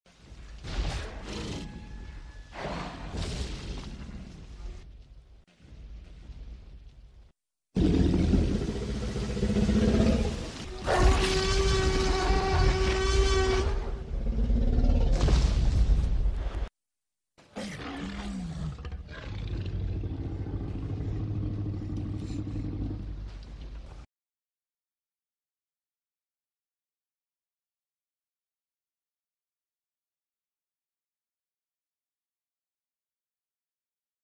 Breakout of Jurassic Park--Made from Video 101--- HD
Description-made from green screen dinosaur from tobler green screen ---creative commons video___original cgi Trex animation done by  Dimitis Galatas -Paranormal enities --modified by Tobler Green Screen--